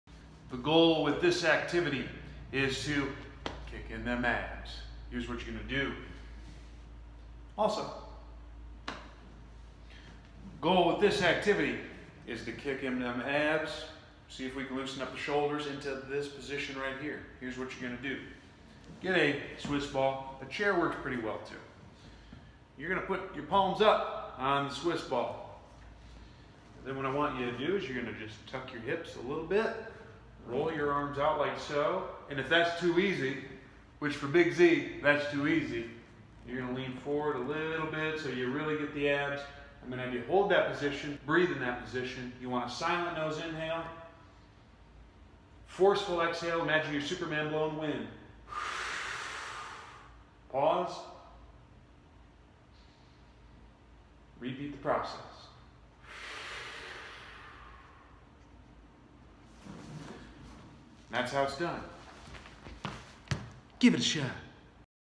Swiss Ball Rollout with External Rotation
Get abs and shoulder ER with this one.